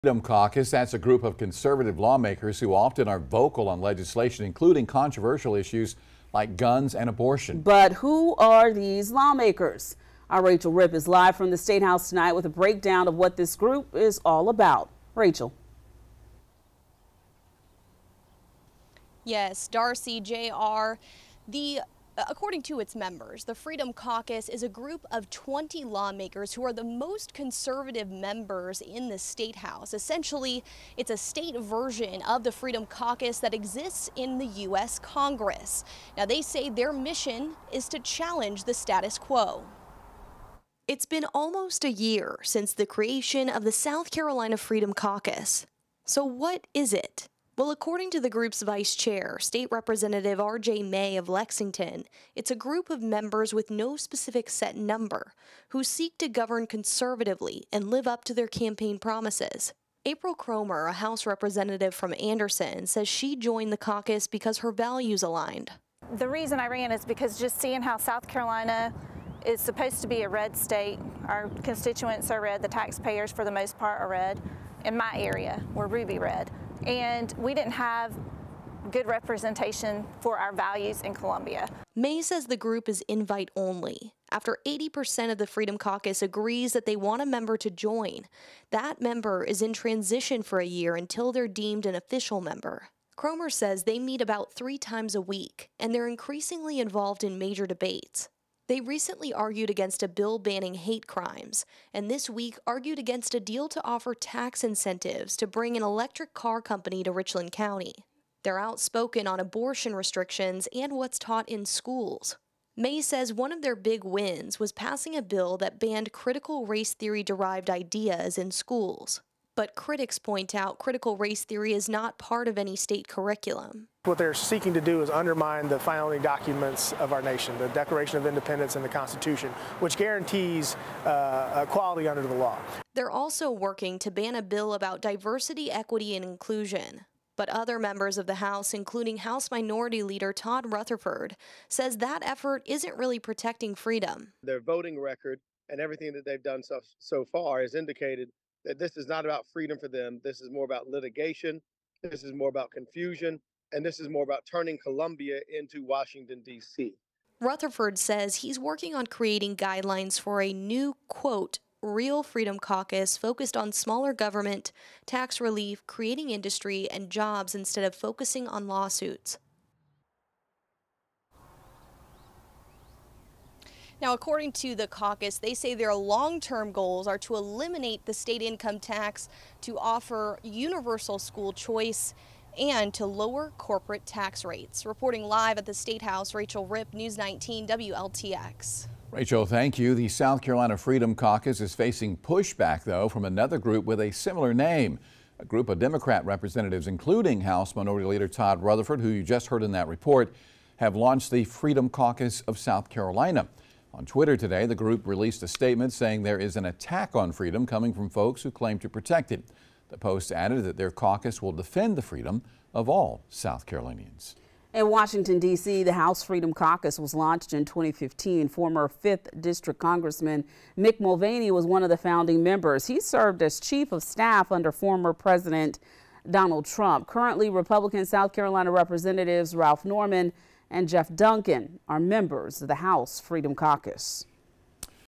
What is the Freedom Caucus?
A group of 20 conservative lawmakers who are vocal on legislation including issues like guns and abortions.